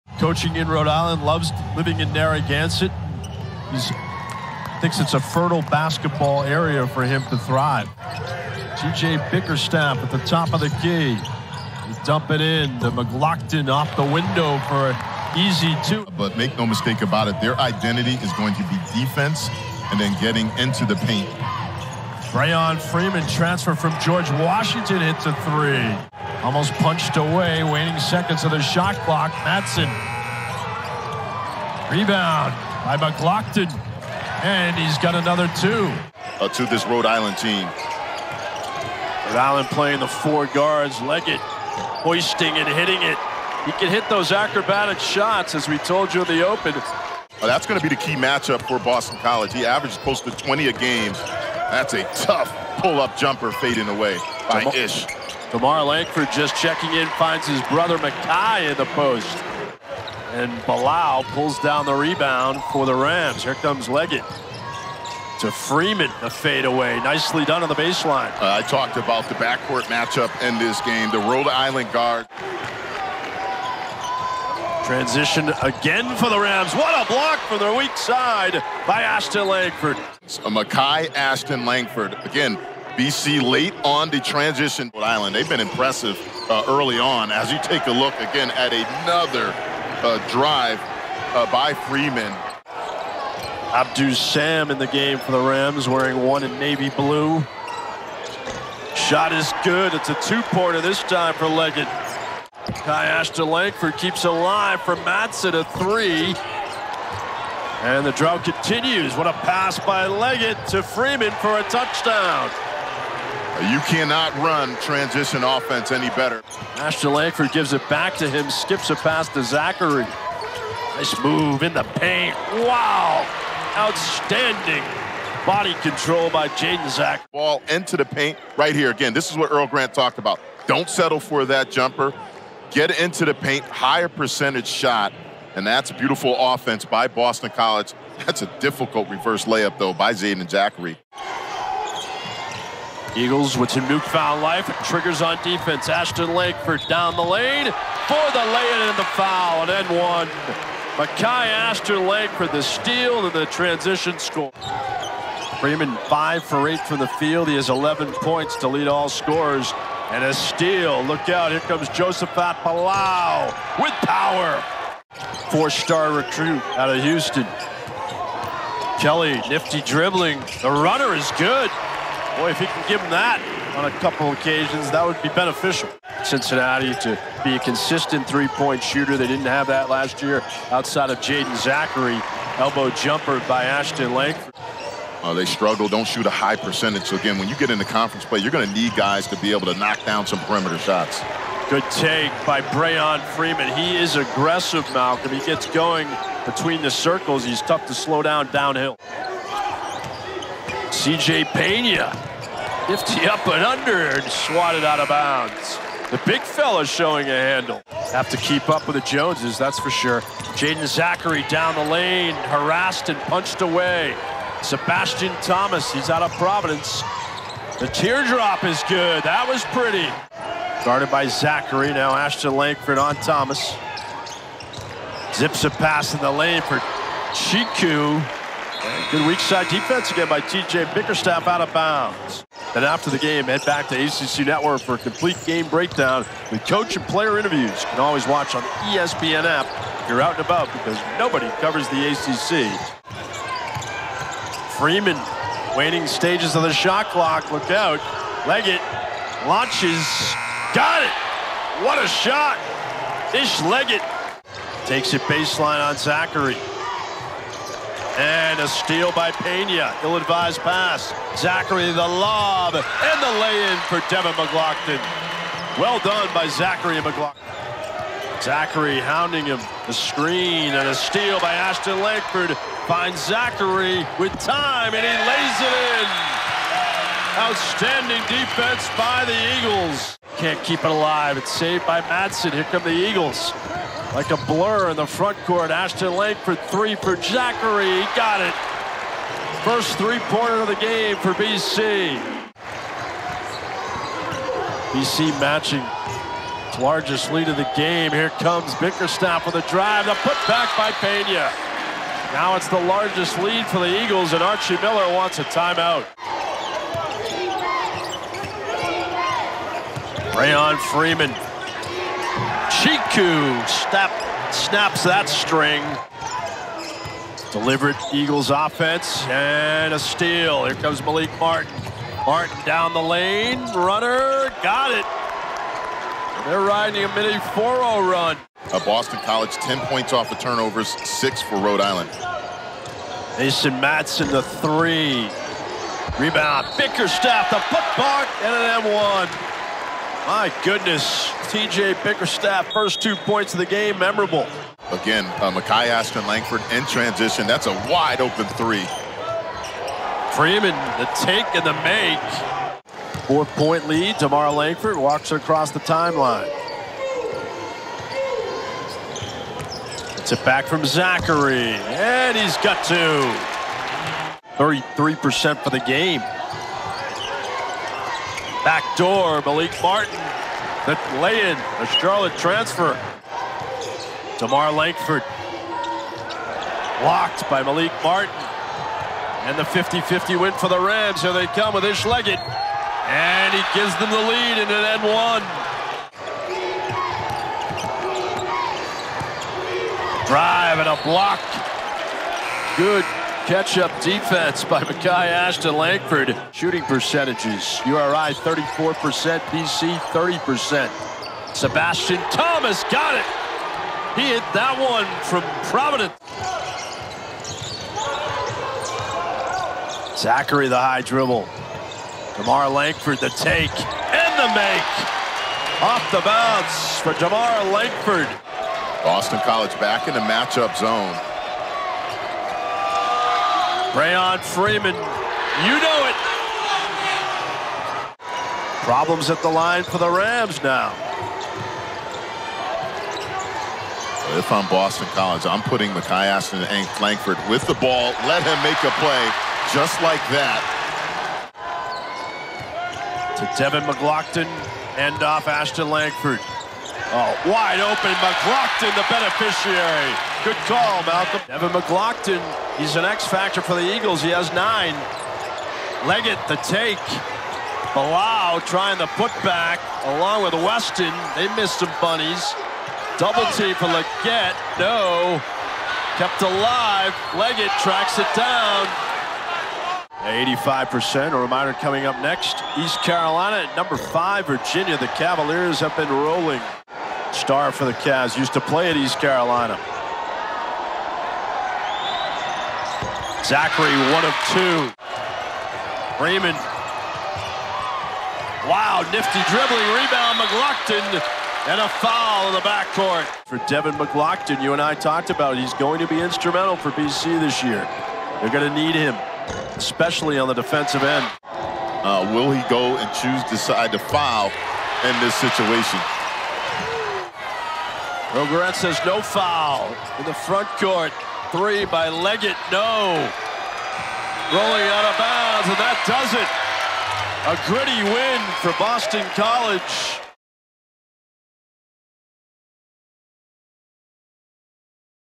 Rhode Island vs. Boston College Condensed Game | 2022-23 ACC Men’s Basketball
Boston College overcame a tough shooting afternoon and used strong defense in a win over Rhode Island, 53-49. A key difference maker for the Eagles was going +5 in the turnover margin and doubling up the Rams in points off turnovers (16). Boston College also held Rhode Island without a field goal for the last five minutes of the game. Jaeden Zackery led the Eagles offensively with 16 points.